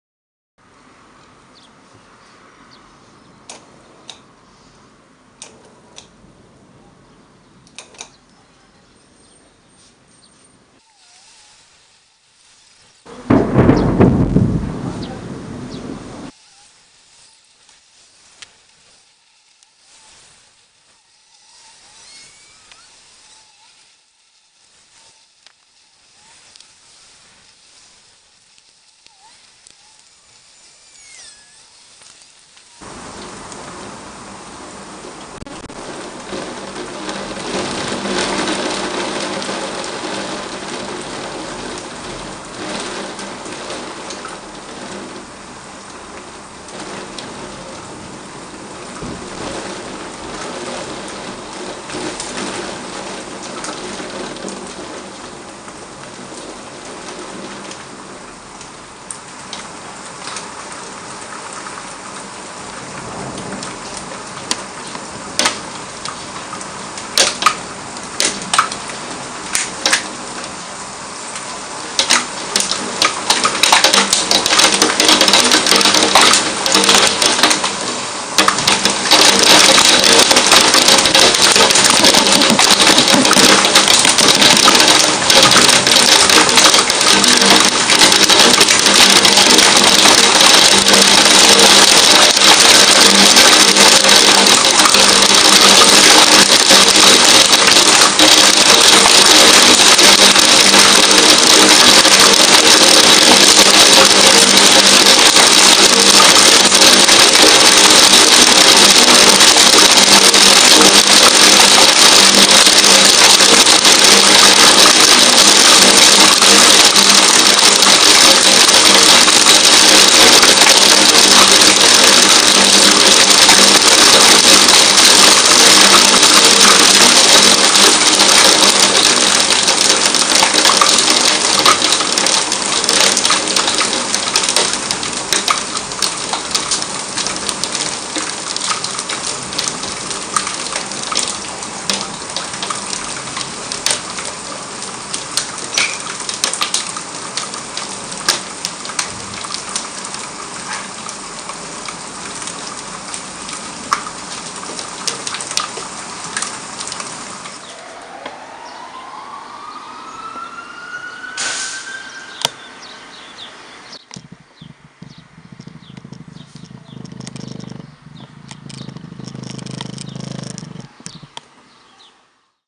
29-6-2014 una tempesta de les bones, a santa maria de palautordera
aquest video tracta de, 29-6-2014  una tempesta de les bones, a santa maria de palautordera